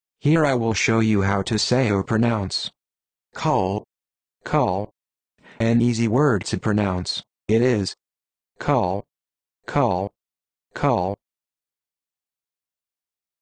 How to Say or Pronounce Cull
This video shows you how to say or pronounce Cull.

How accurate does it say Cull? How would you say Cull?